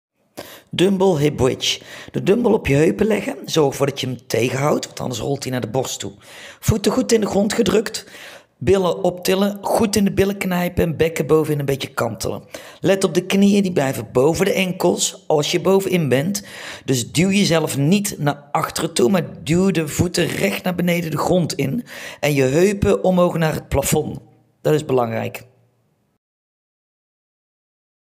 Dumbell hipbridge
Goed knijpen in de billen bovenin, bekken kantelen. Werk niet te licht.